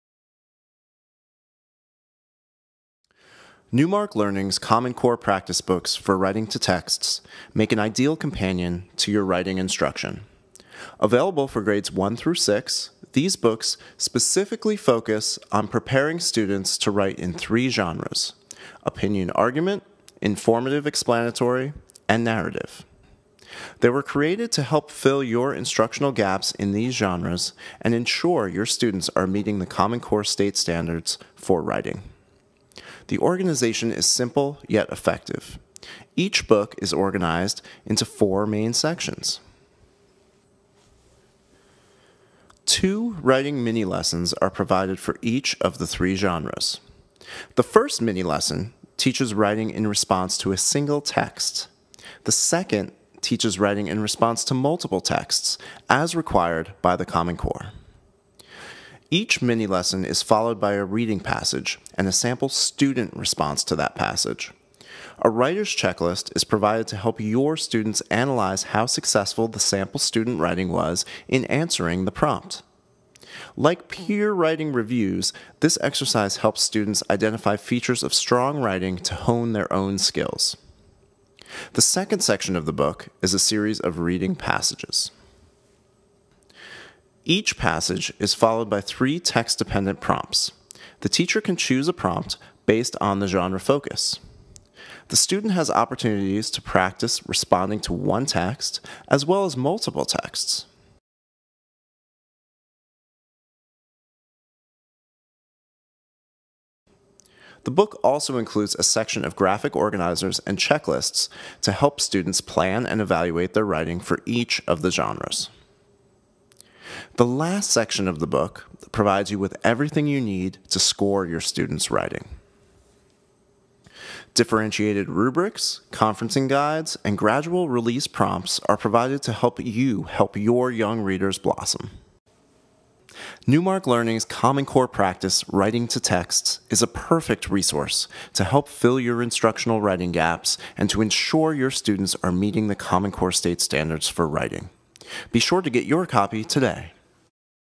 Common Core: Writing to Texts
How can teachers successfully prepare their students for the new text-based writing assessments? Common Core: Writing to Texts titles meets all the Common Core State Standards (CCSS) for writing. The series provides rubrics with differentiated instruction to help every student achieve on-grade-level competency in writing.
The series contains six grade-specific titles for Grades 1-6 and can be used in mainstream classrooms, resource rooms, pull-out intervention settings, after-school programs, and at home.